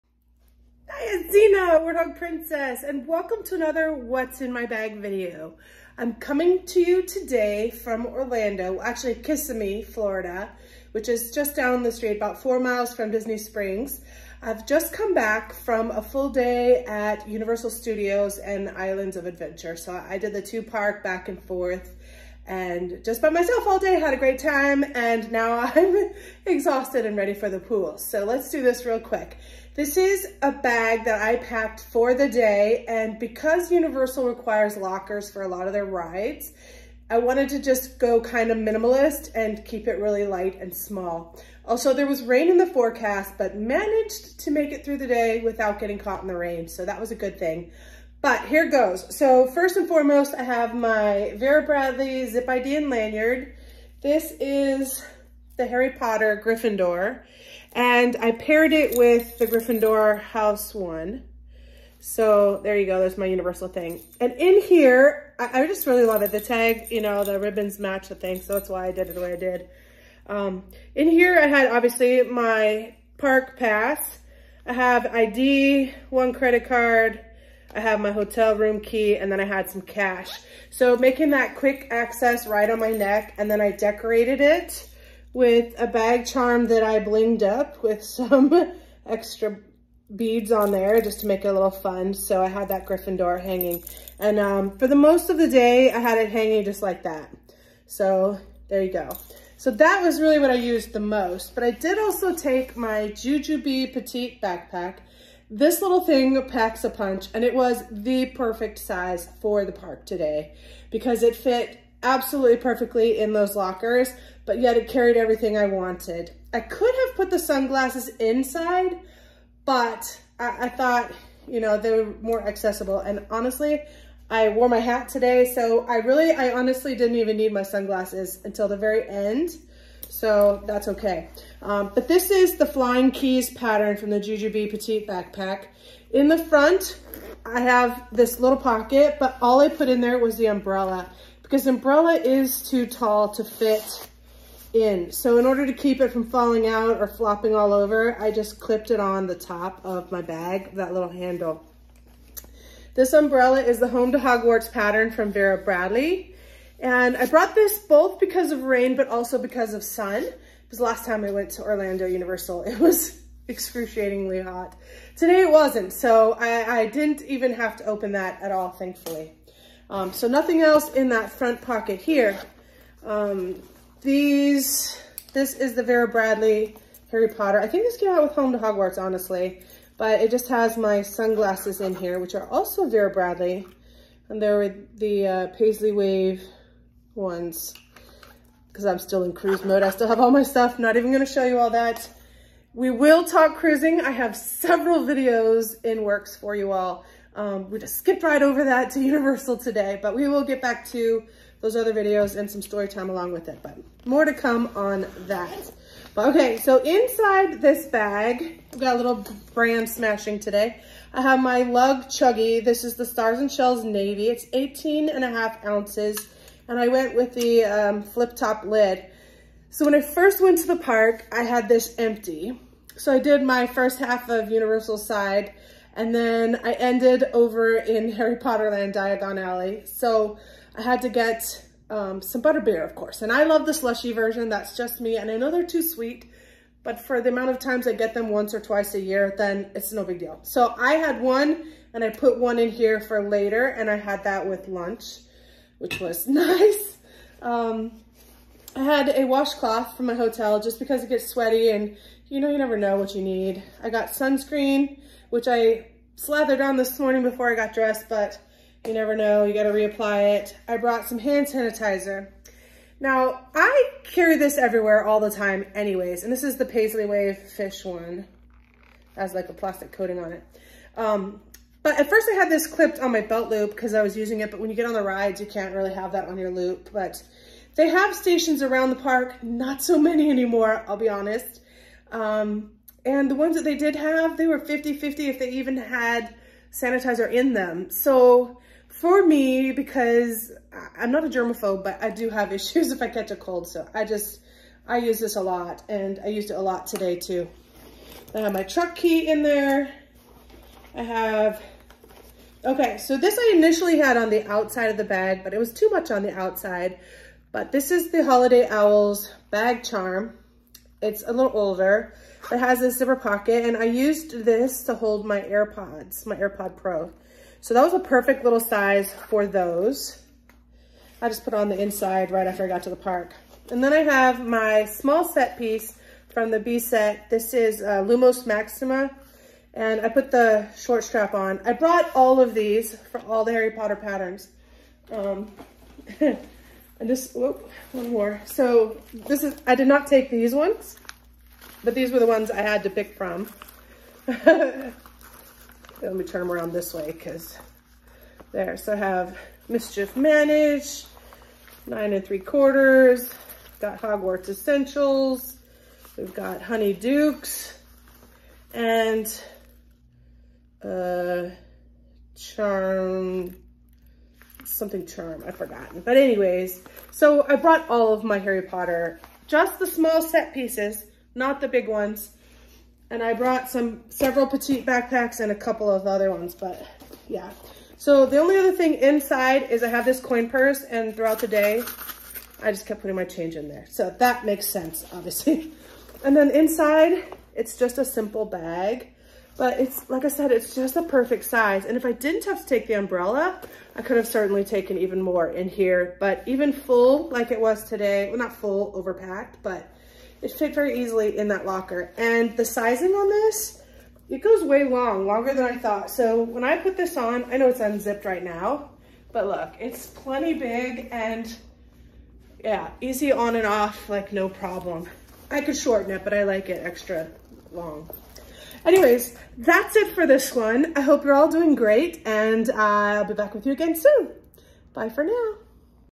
What’s in My Bag — Packed for a Day at Universal Studios Orlando
This what’s in my bag video is a brand smashing of Lug, Jujube and Vera Bradley and features the Jujube Petite Backpack (Flying Keys) and Vera Bradley Gryffindor Lanyard and Zip ID. It is shown as was packed for a day at the parks Universal Studios Orlando & Islands of Adventure.

During the video I forgot to show what was inside the small set piece (Lumos Maxima). It contained a hair elastic, lotion, chapstick, medicine and a face mask.